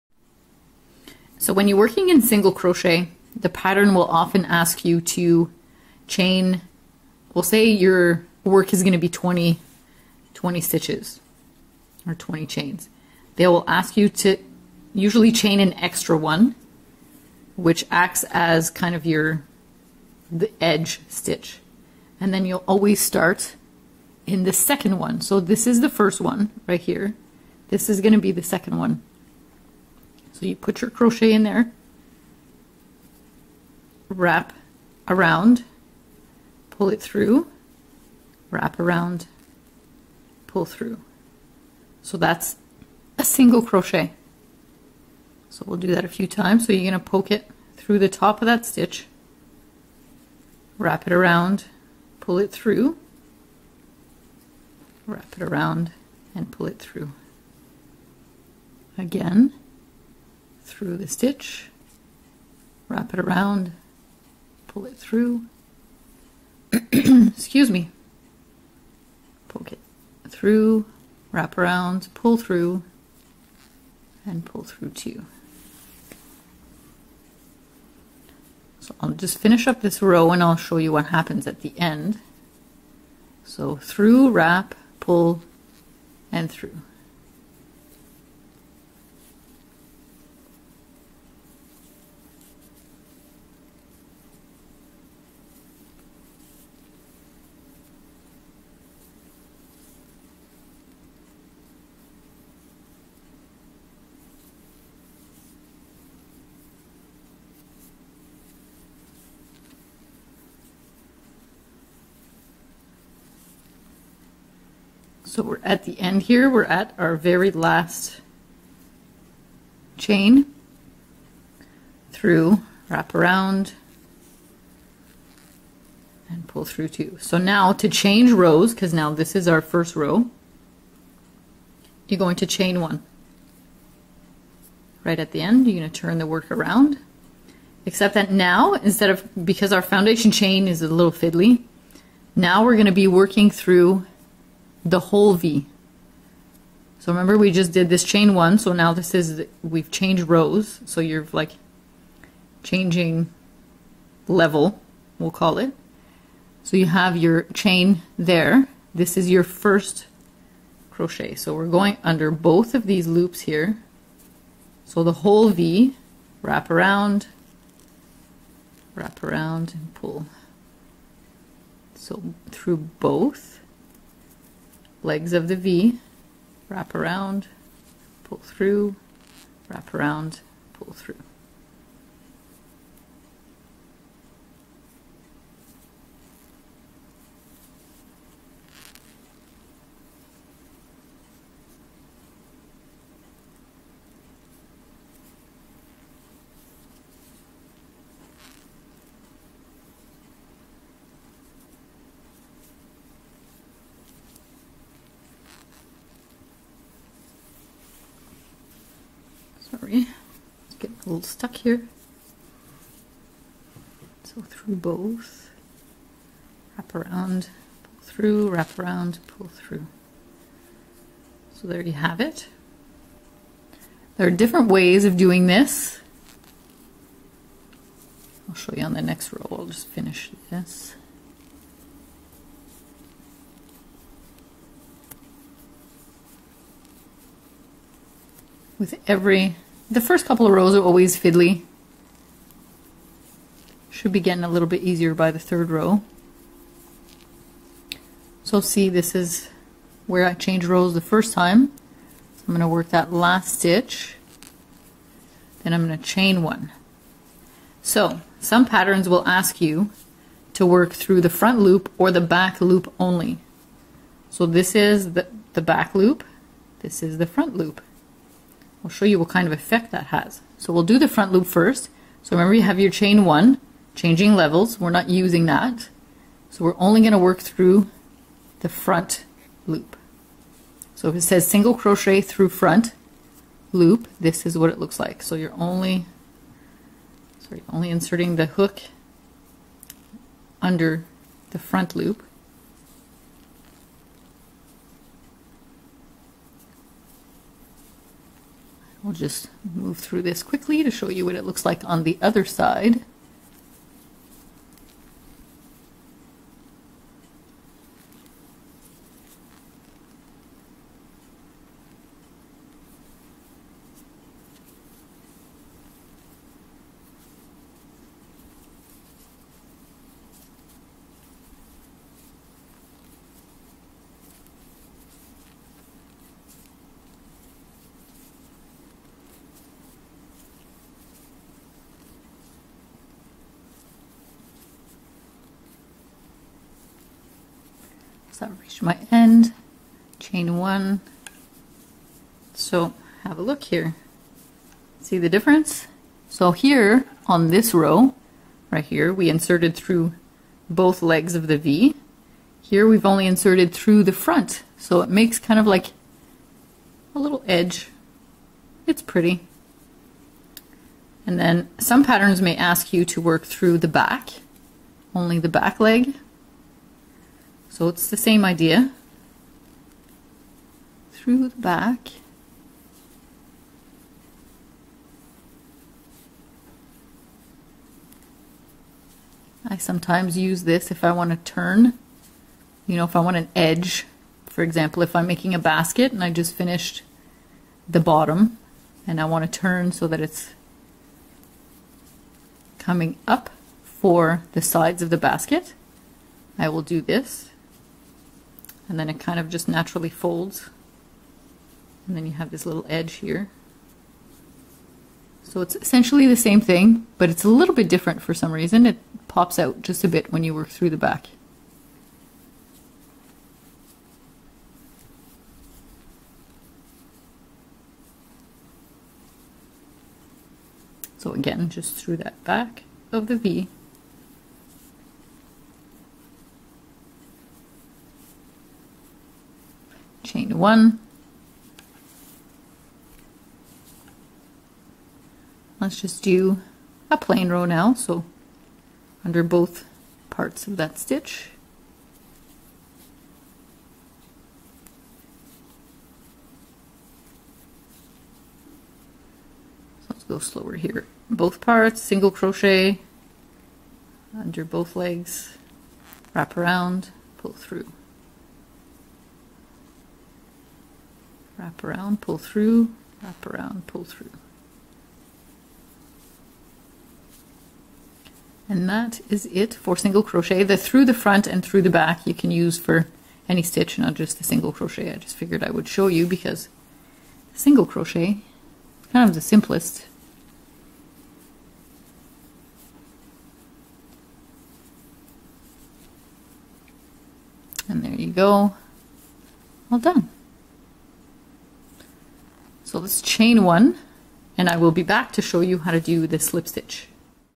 Crochet Basics - Single Crochet
This video shows you how to work a single crochet and how to change rows (levels) in that stitch.